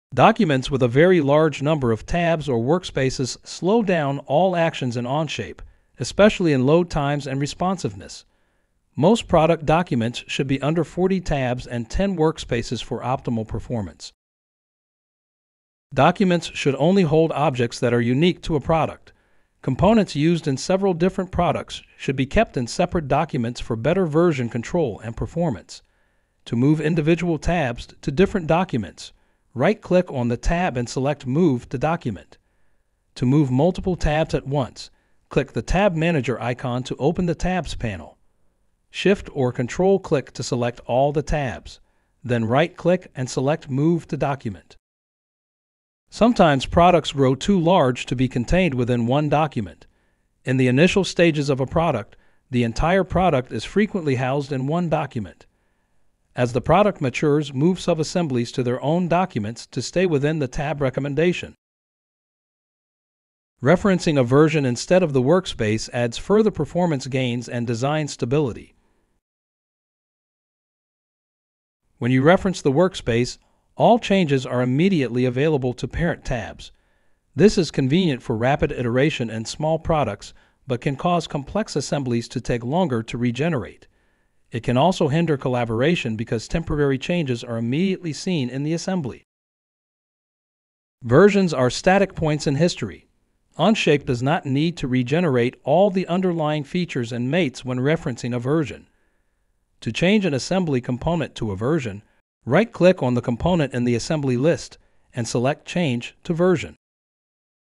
Tips for Organizing Complex Documents
Keep Documents under 40 tabs and 10 workspaces.
Model library components in separate Documents for better version control.
Reference versions instead of workspaces in the Assembly list.